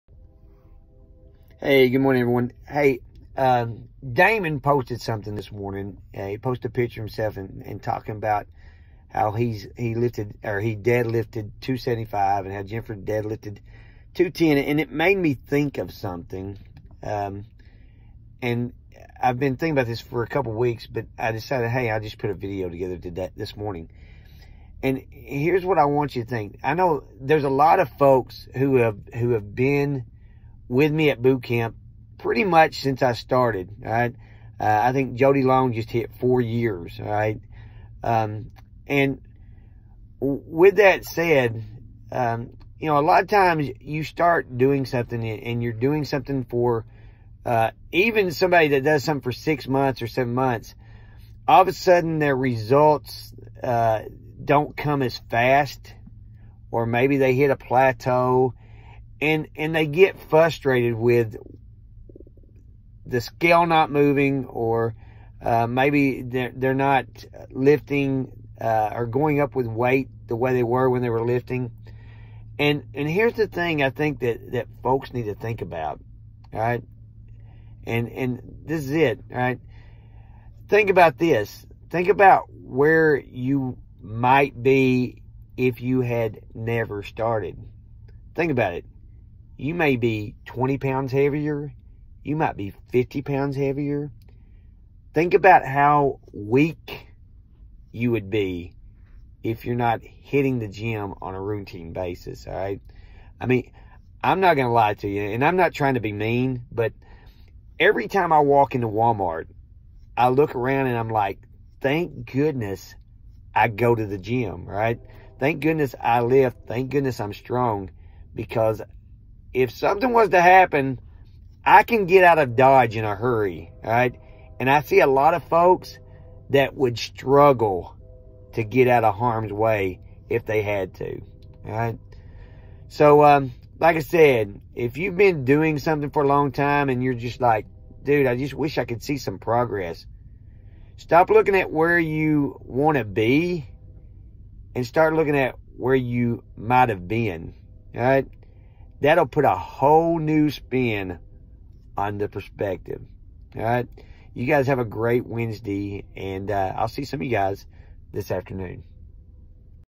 When You Get Frustrated!
A lot of times when you’ve been working out for a while you can get frustrated because you don’t see the same kind of results you seen when you started.

I ask you to think about it differently today. Imagine where you might be if you hadn’t started. Would you be 20 lbs or 50 lbs heavier?  Could you lift that bag of quickcrete?  Could you run 50 yards to get out of harms way.

I’m not trying to be mean but every time I walk through Walmart I’m thankful I hit the gym yesterday.

Sometimes it helps to think about where you might have been versus the fact you can’t lose those last 10 lbs.